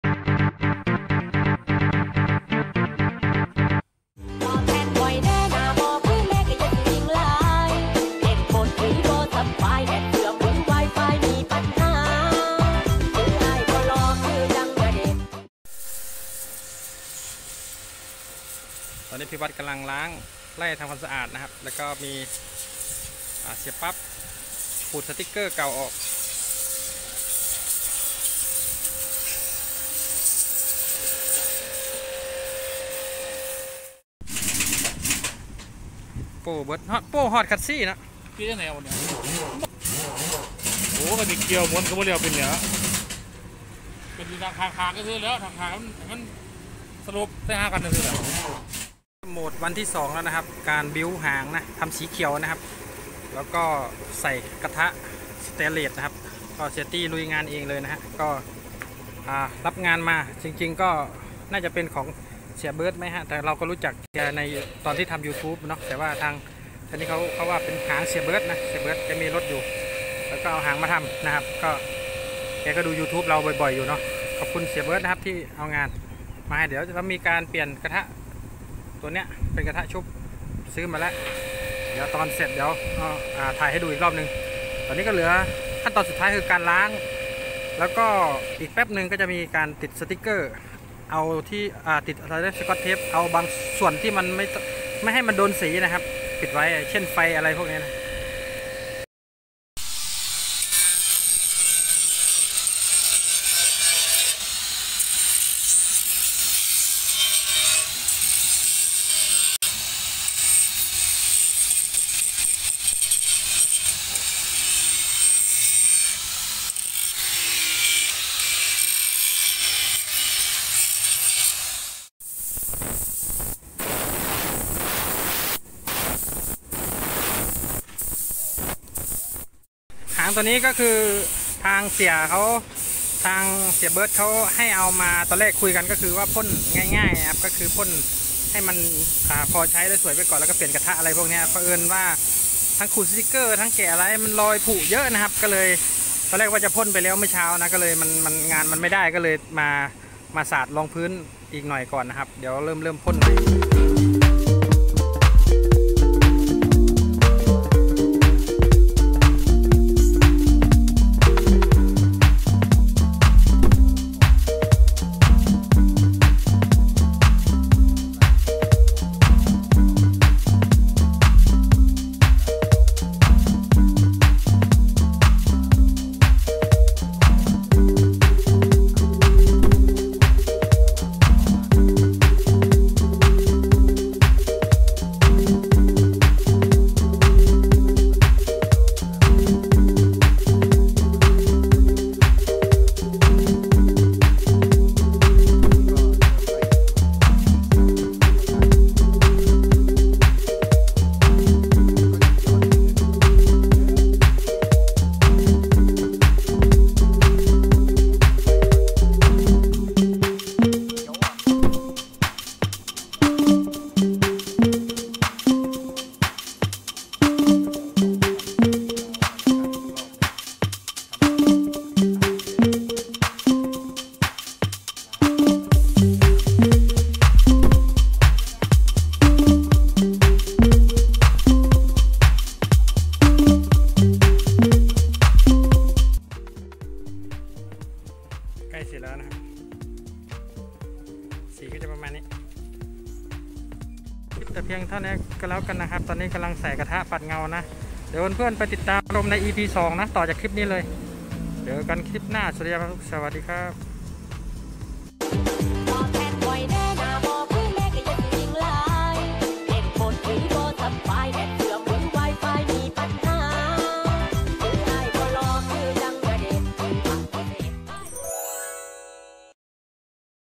หางเก่านำมาบิ้วทำสีเขียวใหม่EP1.
คลิปนี้พาดูหางที่เก่ามากแล้วมาเปลี่ยนกะทะชุบล้างขูดแกะทำสีใหม่